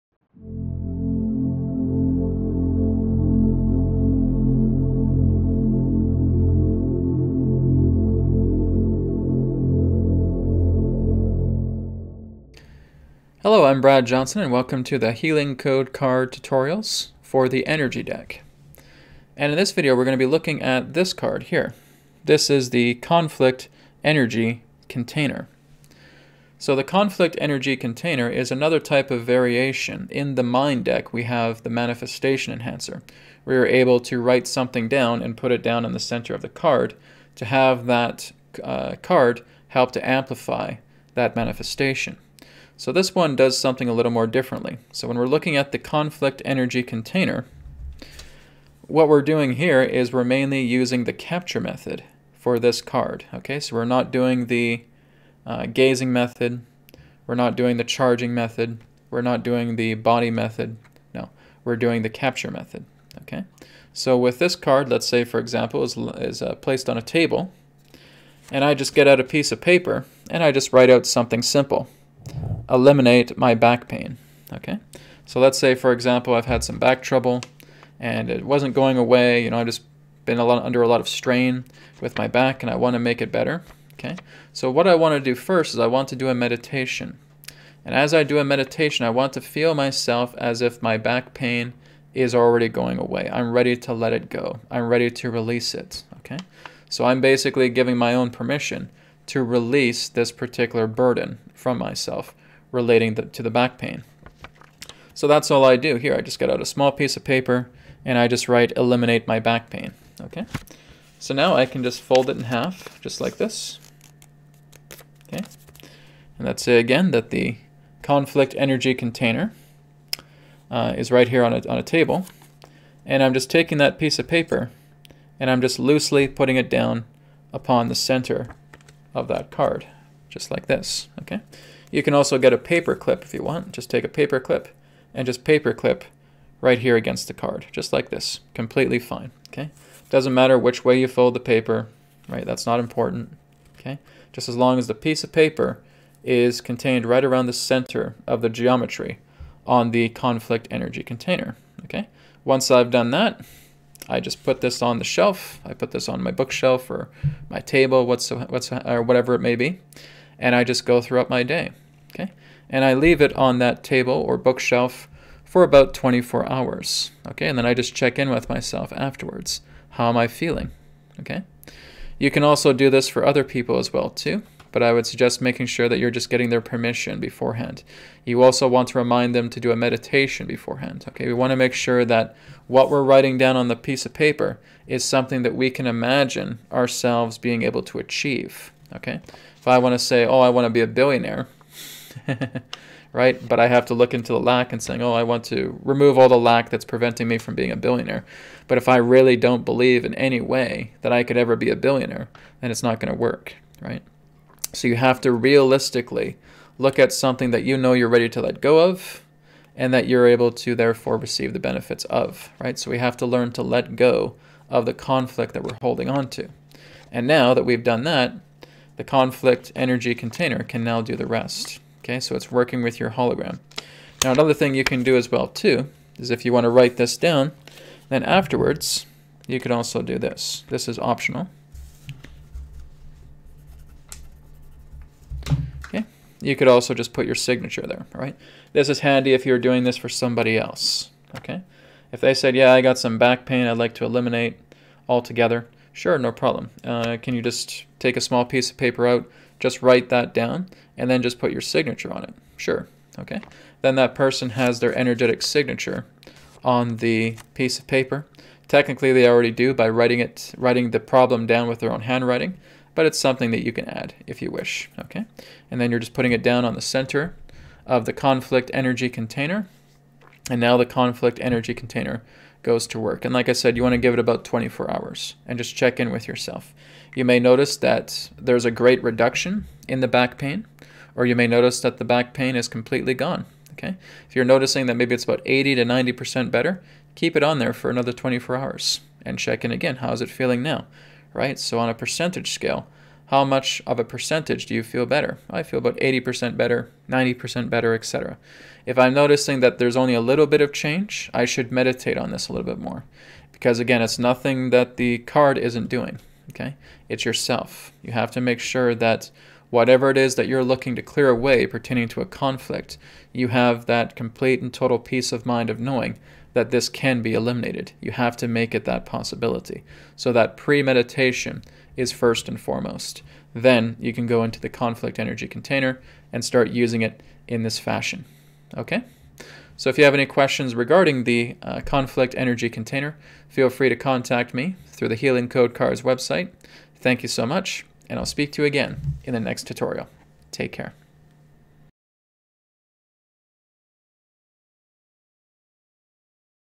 The Conflict Energy Container Card: Energy Deck (Tutorial)
This is a video tutorial that looks into the functionality of the Conflict Energy Container card in the Energy Deck.

The third in the series of the Healing Code Cards. The Energy Deck is a purification, rejuvenation and optimization card deck with Spirit imbued healing codes are contained within the geometric designs of each card. Every card is specifically coded to assist with specific areas of the body to assist with personal self-healing.